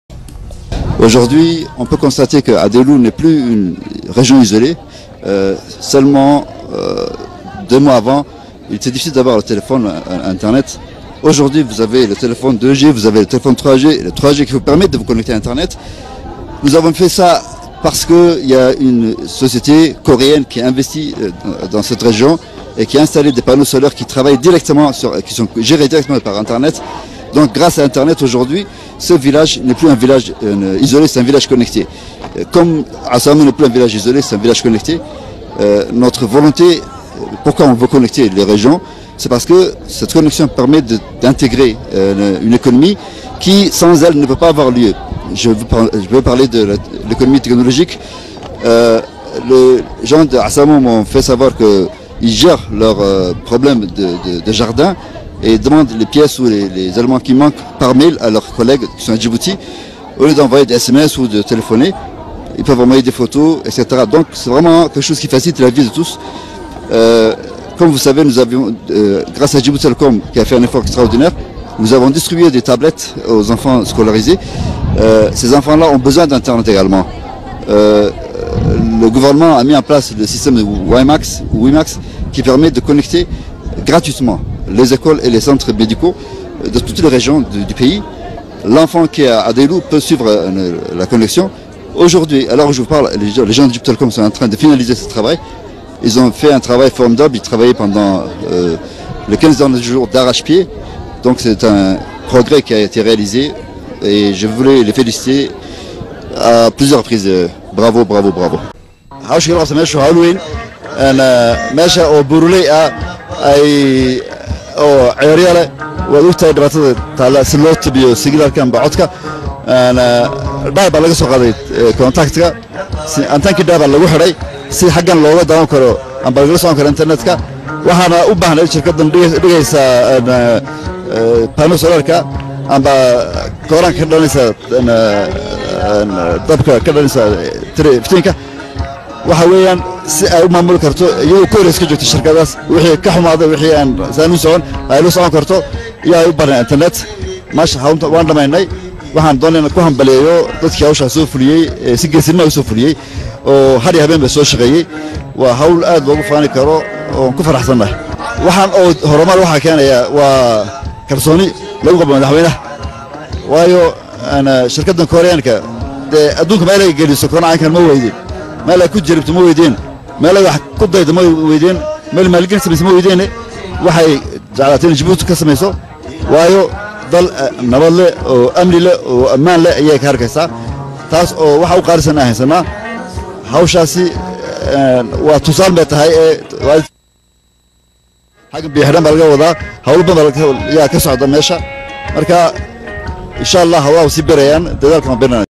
#‎Djibouti‬ ‪#‎SolaireAdaylou‬ Interview du Ministre des Télécommunication très instructif, Une cent
#‎Djibouti‬ ‪#‎SolaireAdaylou‬ Interview du Ministre des Télécommunication très instructif, Une centrale Photovoltaïque contrôlé et orienté grâce à l’internet‪#‎MERN‬ Interview en Français et en Somali